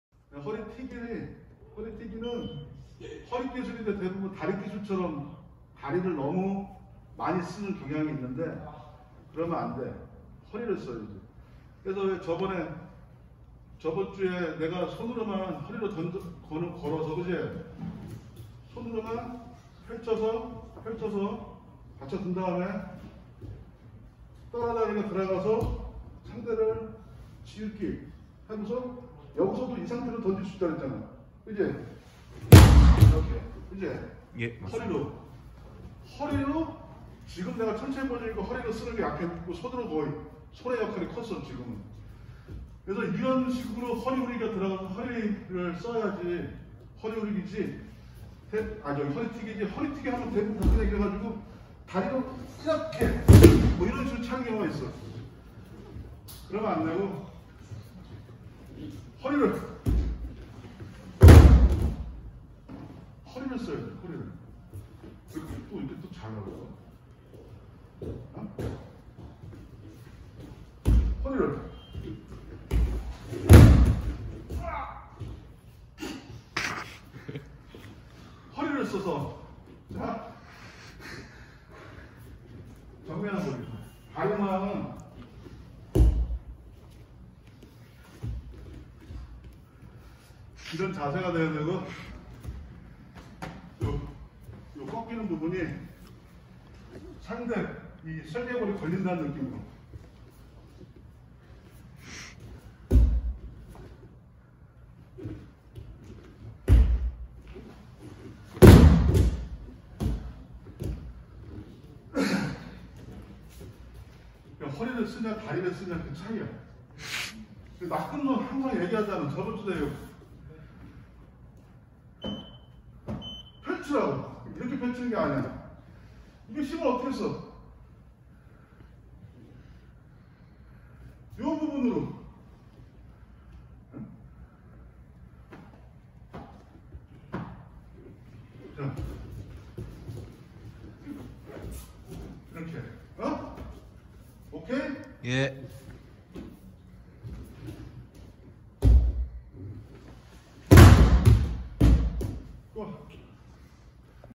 허리튀기 ( 제1장 ) , Hane-goshi
허리튀기 (  제1장  )

허라튀기는 허리기술이지만 다리기술인 허벅다리걸기와 차이가 없이 다리로 던지는 경향이 많다.

허리튀기는 과거 일본 유도가 한명이 영국에서 유도 지도를 하다 개발한 기술로 자신의 특기 기술인 허벅다리걸기를 할 수 없는 부상으로 만들어진 기술이라고 전해진다.

그래서 허벅다리걸기와 매우 흡사하지만, 우리식 기술명에서도 전달하는 기술의 원리는 튀기이다. 허리로 튀긴다는게 중요하다.

[ 일화 ]
허벅다리걸기에 한걸음 허벅다리걸기라고 하며 원스텝으로 던지는 방법이 있다, 이 방법에 대해서 해외에서도 허벅다리걸기인지 허리튀기인지를 잘 모른다

나는 한걸음 허벅다리걸기에 대해서 허리튀기 처럼 걸수는 있으나 정확하게는 허벅다리걸기가 맞다고 본다, 한걸음, 한번의 스탭으로 거둬 차주는 부위가 결국 상대의 허벅지 안쪽으로 공격자의 다리가 들어가 차올리기 때문이다

그래서 한걸음 허벅다리걸기는 정확하게 말하자면? 한걸음 큰허벅다리걸기이다.